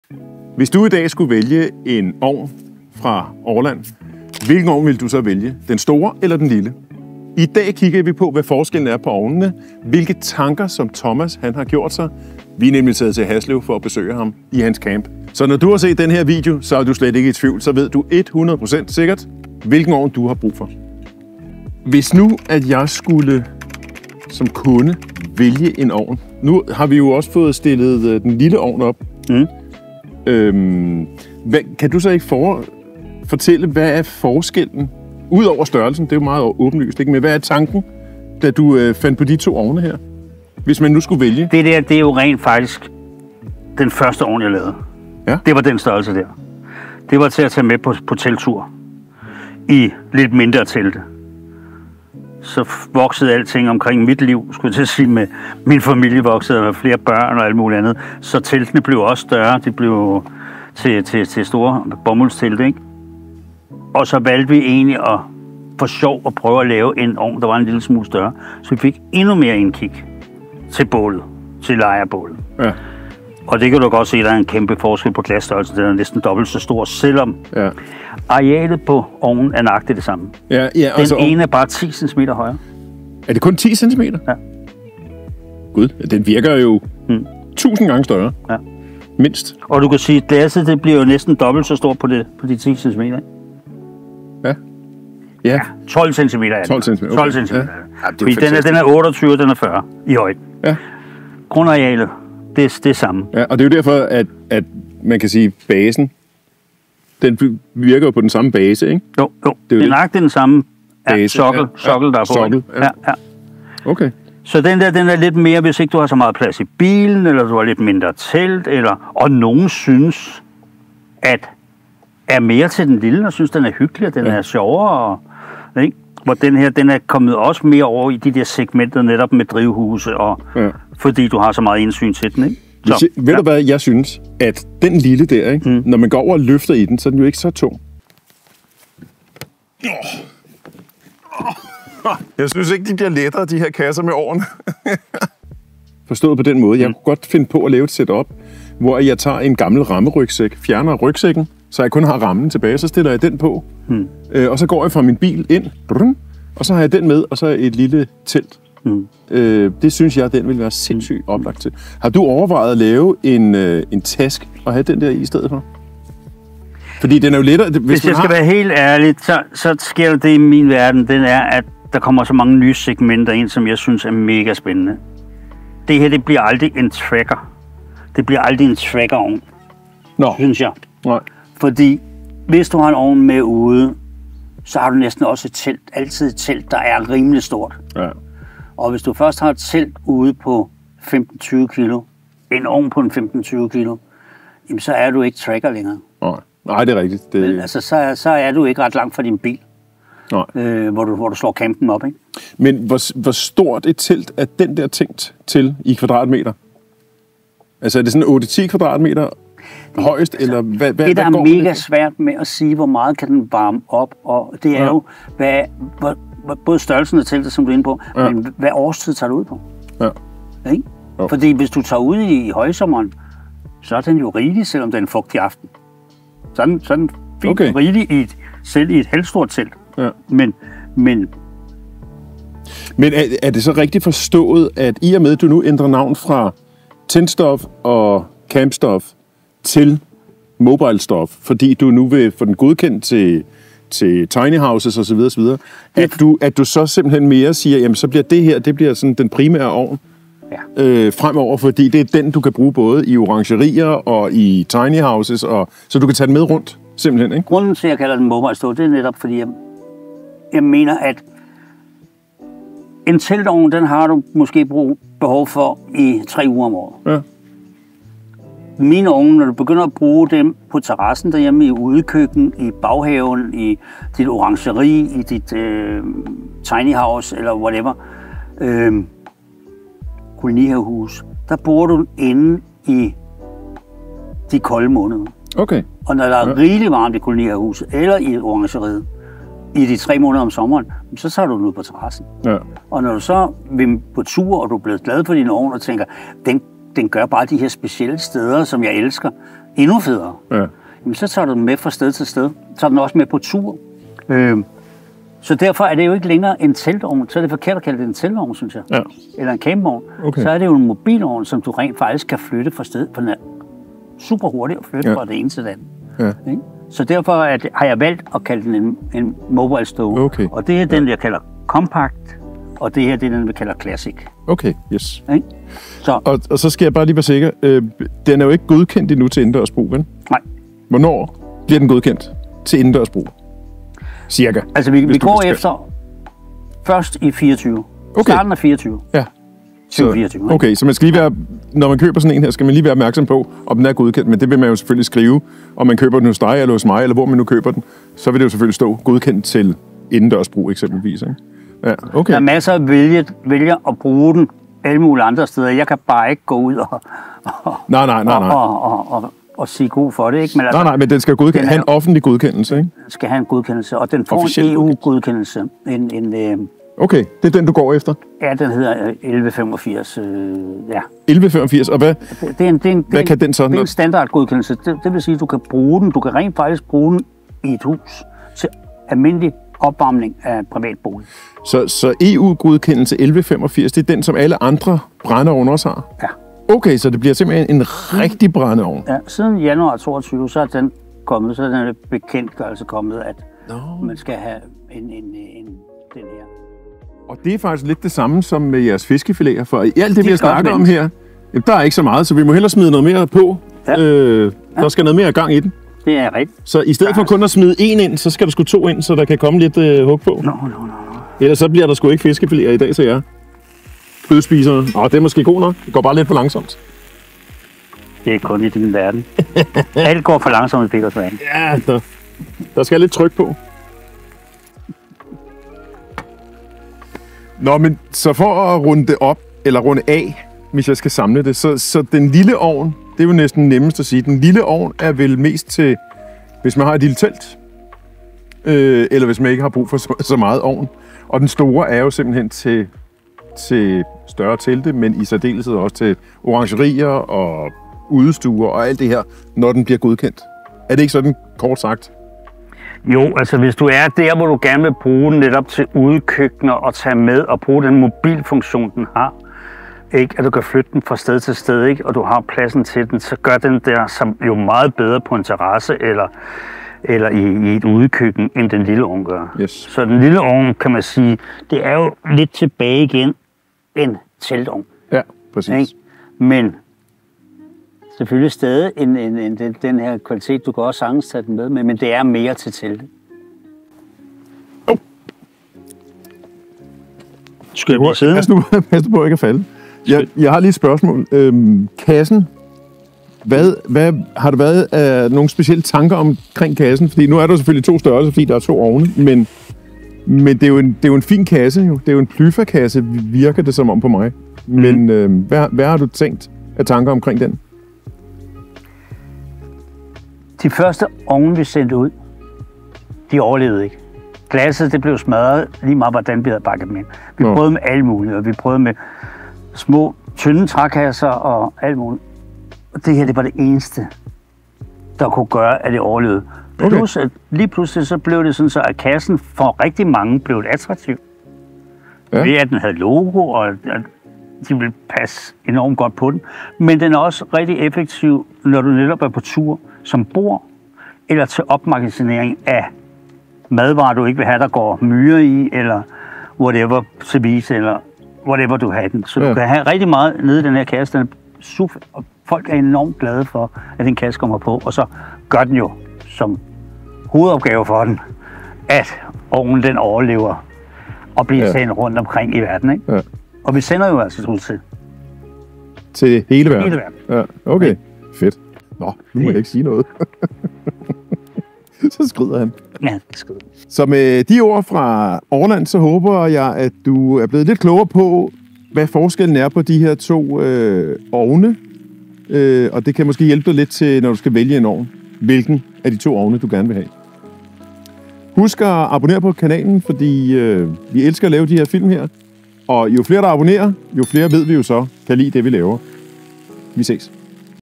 Kan en Orland ovn bruges lovligt i et hus?
Det at kunne se flammerne fra 3 sider, har gjort Olands ovne meget populære.
Vi er taget ned til Thomas Orland's camp for at snakke om forskellen mellem de 2 ovne og vi snakker om tankerne bag ovnene.

Det store spørgsmål om ovnene kan blive godkendt til eksempelvis campingtelte, orangeri, tinyhouses, eller måske indendørs i et hus, snakker vi også om.